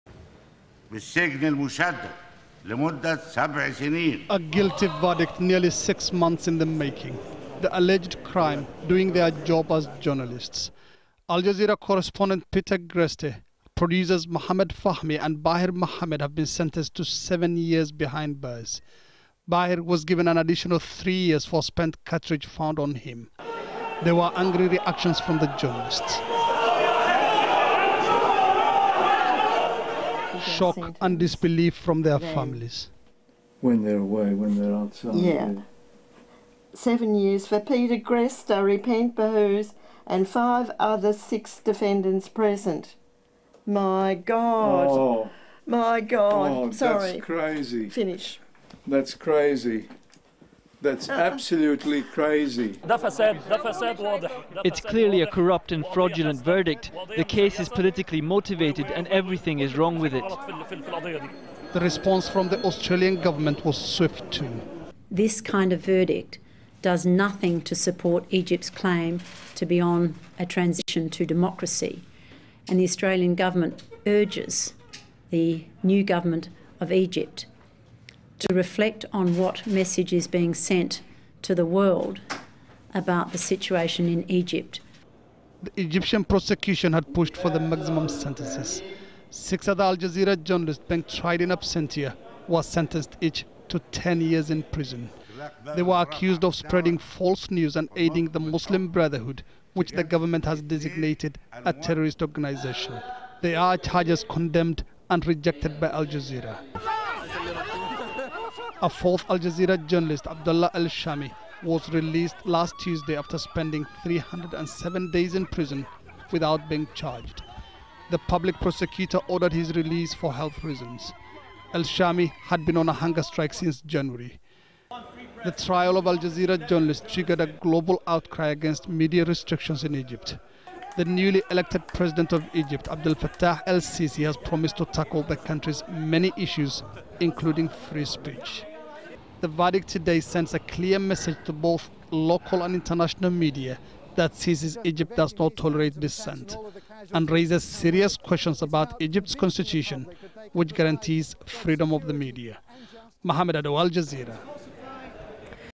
Egypt court sentences Al Jazeera journalists
Two Al Jazeera English journalists have been sentenced to seven years in jail and one to 10 years by an Egyptian court on charges including aiding the Muslim Brotherhood and reporting false news. The guilty verdicts were announced by a judge on Monday against Peter Greste, Mohamed Fahmy, and Baher Mohamed. Al Jazeera's Mohammed Adow reports.